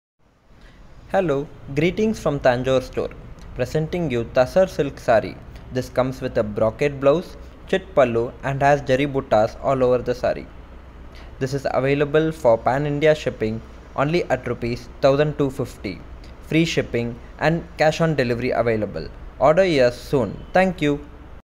Yellow Tussar silk Saree
Yellow Tussar silk saree comes with brocade blouse, Chit pallu and jari buttas all over saree.

Only at Rs 1250.

You can order at: Tanjorestore.com